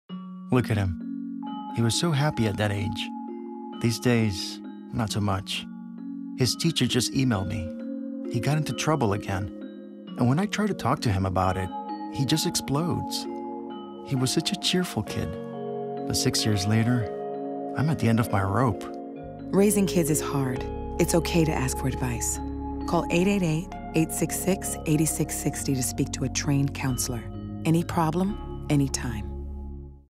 He was such a cheerful kid.
Look at him. He was so happy at that age.
These days... not so much.

Nebraska Family Helpline TV 2015